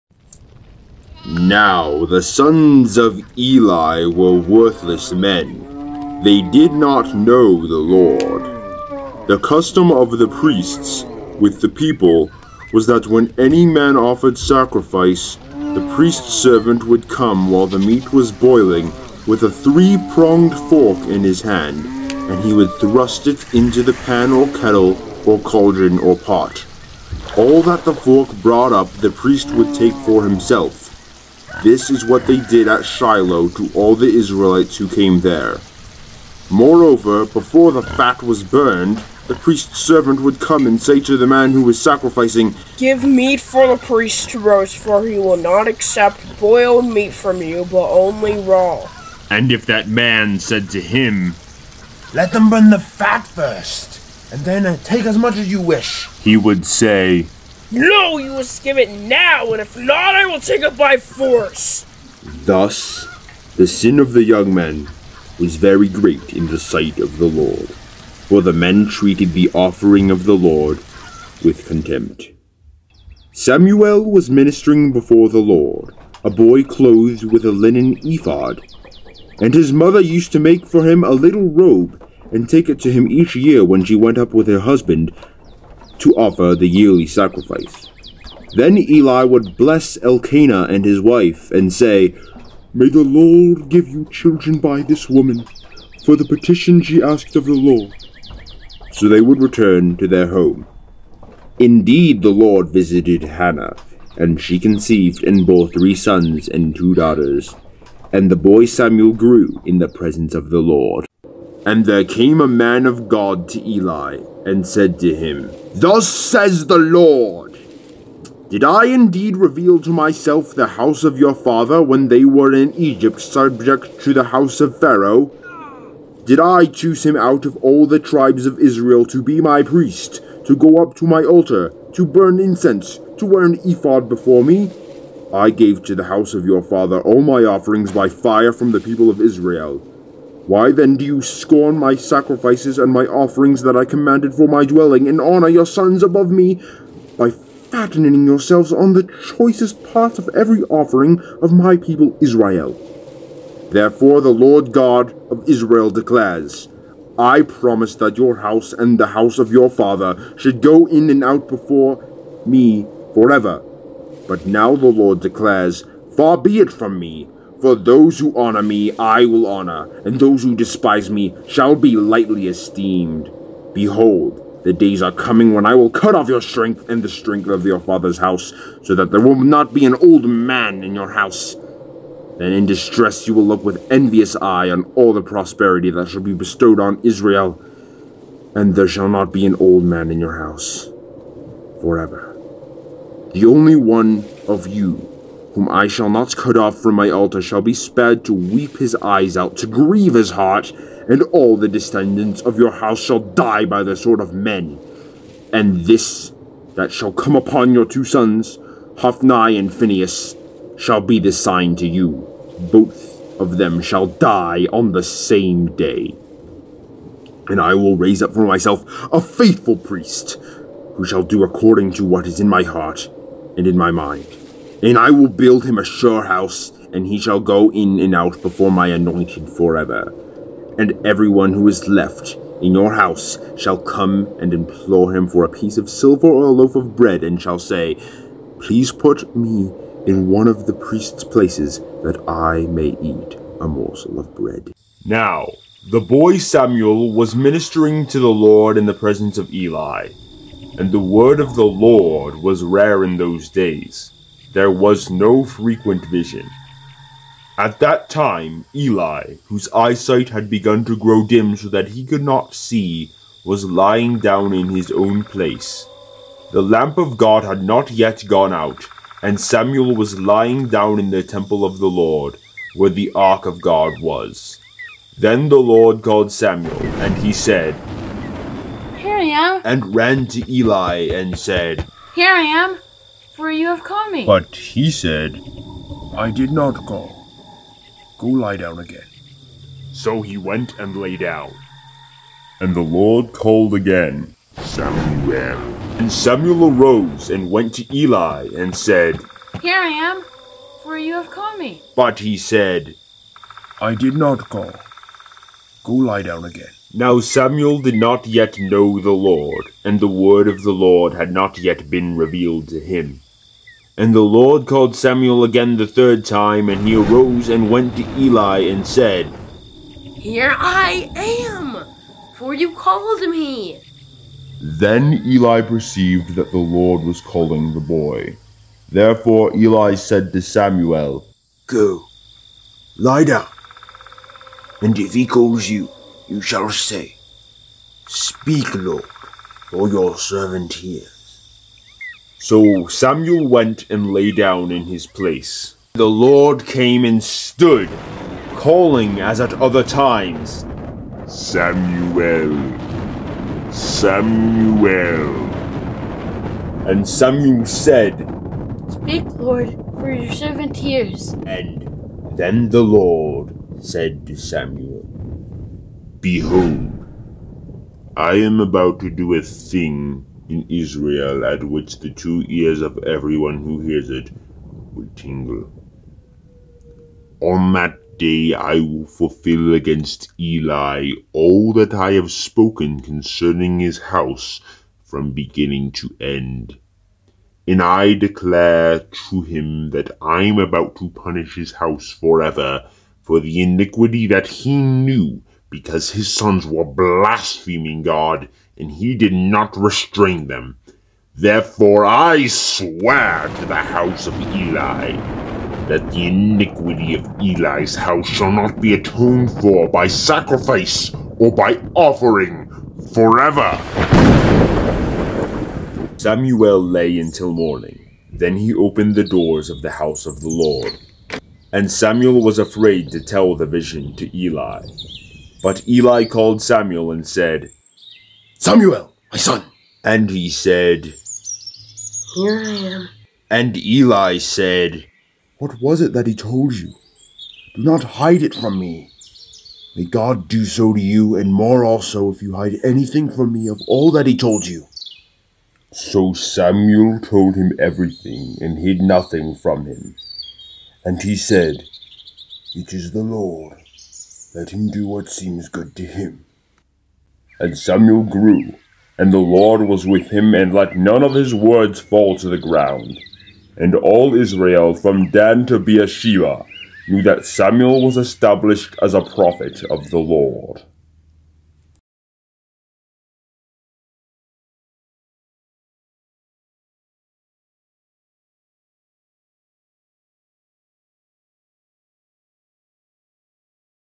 1 Samuel 2:12 - 3:20 | Bible Audio Dramatization | ESV
DIRECTOR                                 Elisha Mummert

EXECUTIVE PRODUCER.       Elisha Mummert

ELI'S SONS                                Silas Mummert

ELI'S SON'S SERVANT            Silas Mummert

ELI                                               Elisha Mummert

MAN                                           Elisha Mummert

GOD  (I do not claim diety)    Elisha Mummert

SAMUEL                                    Branch Mummert

Background sounds made with Capcut